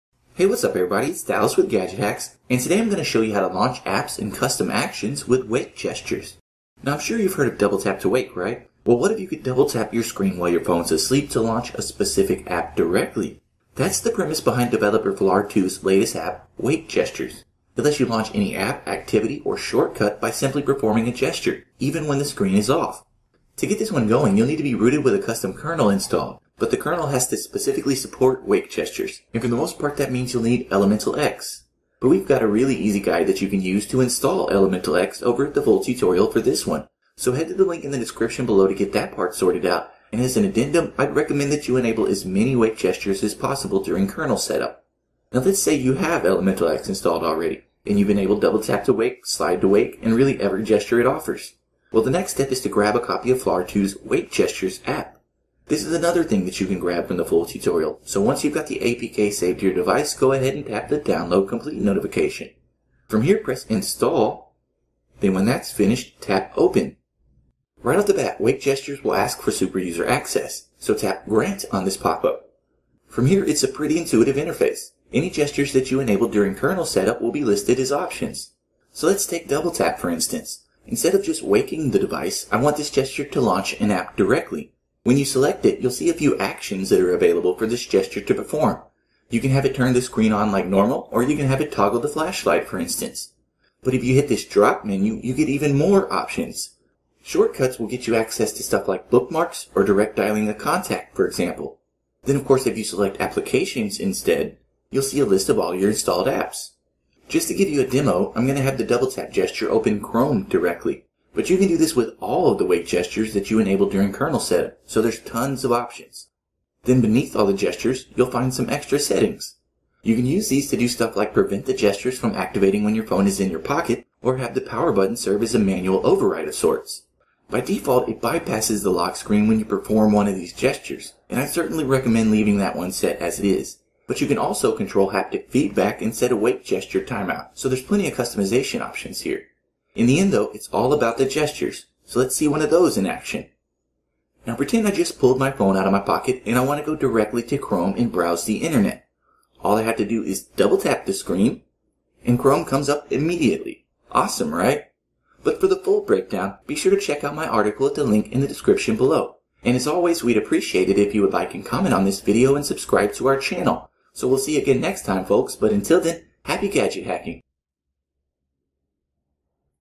Launch Apps & Custom Actions with Wake Gestures [How-To]
How to Customize Wake Gestures on Android

In this tutorial, I'll be showing you how to launch any app, activity, or action with double-tap and swipe to wake gestures. You'll need to have the ElementalX kernel installed to get the wake gestures in the first place, but this root app allows you to assign custom actions to any of the gestures. To find out if your device can use this mod, check this